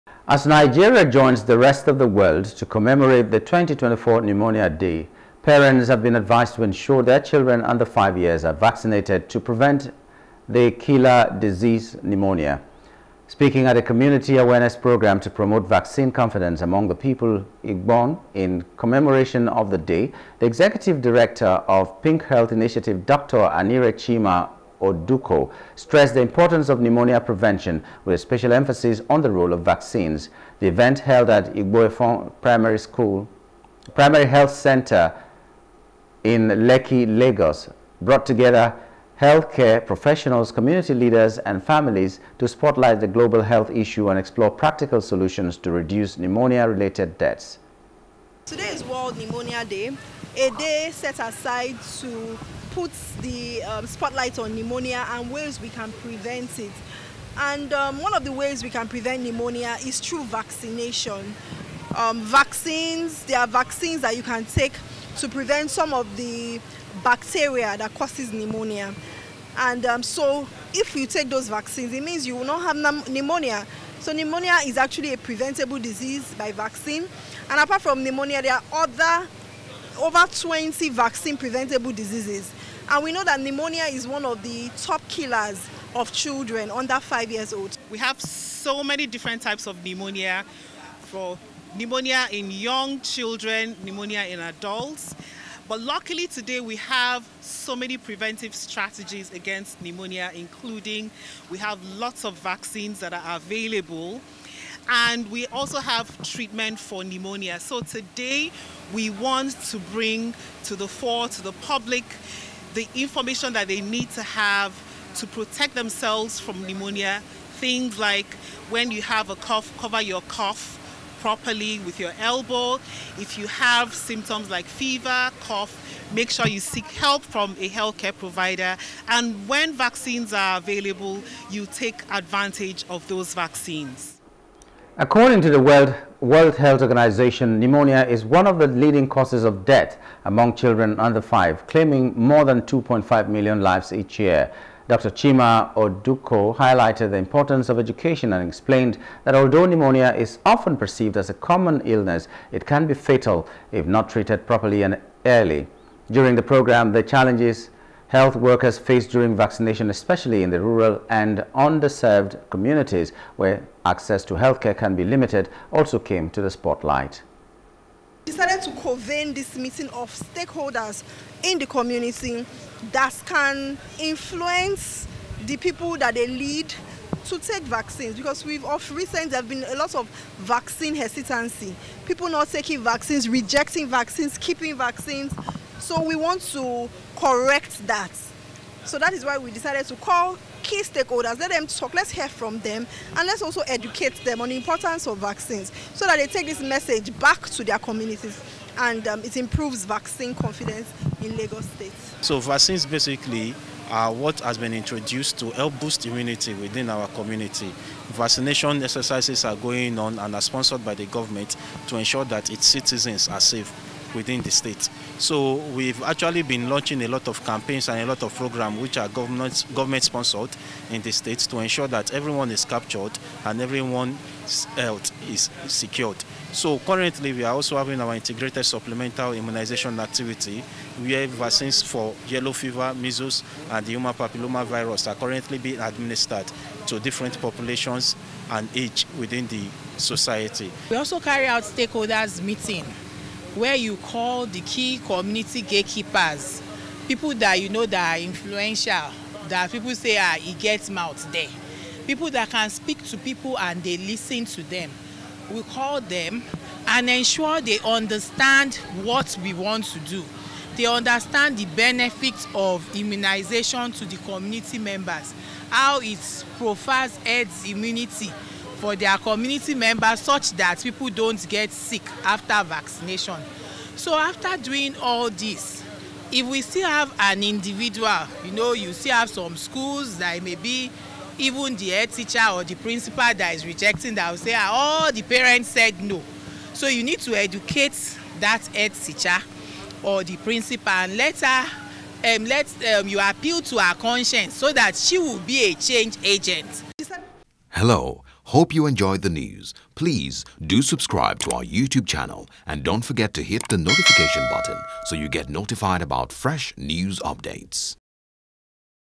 World Pneumonia Day: Medical Experts Advocate Vaccination Of Children Under Five
As Nigeria joins the rest of the world to commemorate the 2024 Pneumonia Day, parents have been advised to ensure their children under 5 years are vaccinated to protect against the killer disease,  pneumonia.
Speaking at a community awareness programme to promote vaccine confidence among the people of Igbon in commemoration of the day, the Executive Director of Pink Health Initiative, Dr Anire  Chima Oduko, stressed the importance of pneumonia prevention, with a special emphasis on the role of vaccines.
The event, held at Igbo Efon primary healthcare centre in Lekki, Lagos, brought together healthcare professionals, community leaders, and families to spotlight the global health issue and explore practical solutions to reduce pneumonia-related deaths.